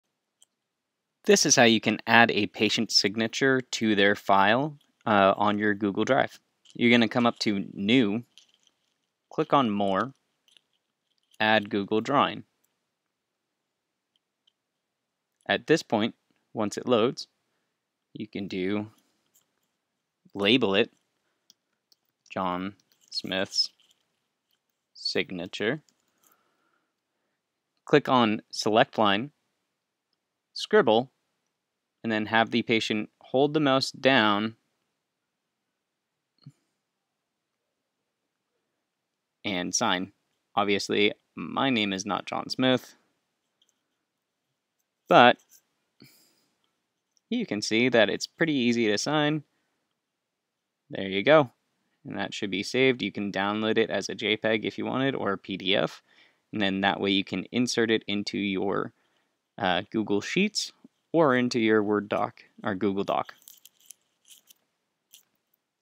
Add Patient or Provider Signature To Google Drive
This video shows you how to add a patient signature or a providers signature to google drive.

If you would like to join the discussion on how to reduce your monthly costs as a healthcare business owner and request more how to videos please click this link below.